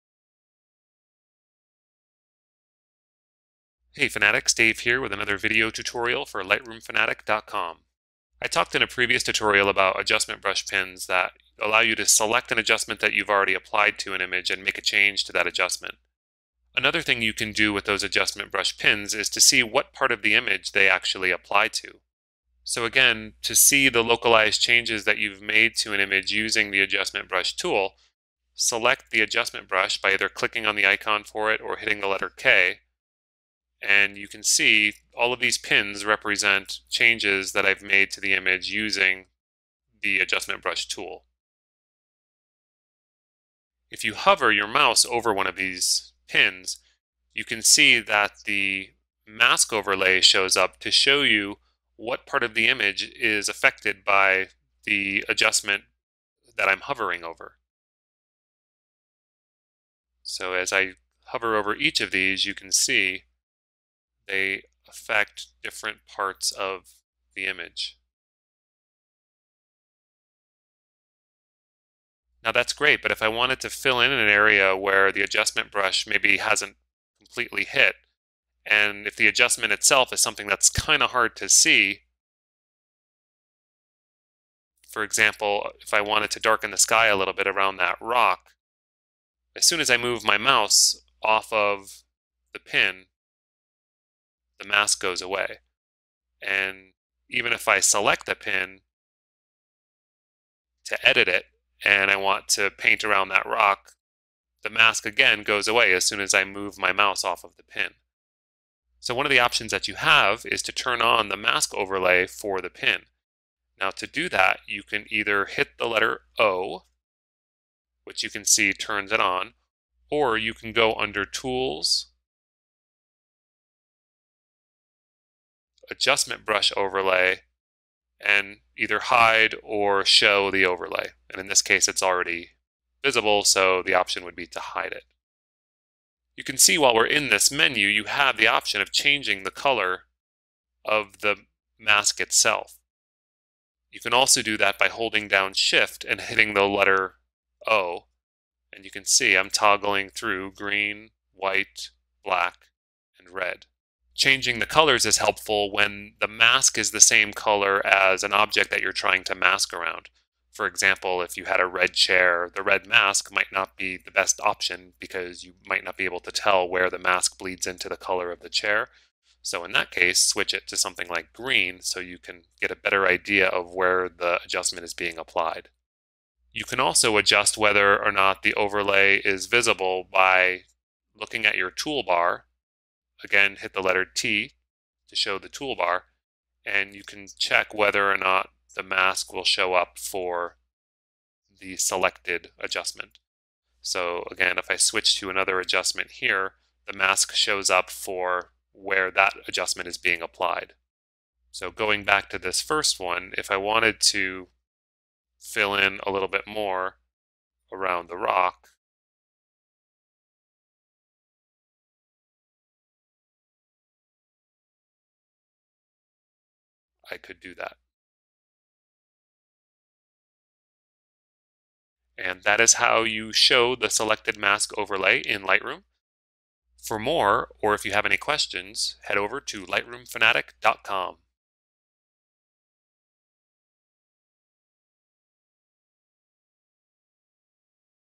Lightroom Fanatic - Using Adobe Lightroom Adjustment Mask Overlay
Especially when an adjustment is faint or hard to see, the mask overlay will help you see what areas of an image are affected by an adjustment, ensuring that your local adjustments stay local and are applied exactly where you want them.

Like this video? Give it a thumbs up or a comment!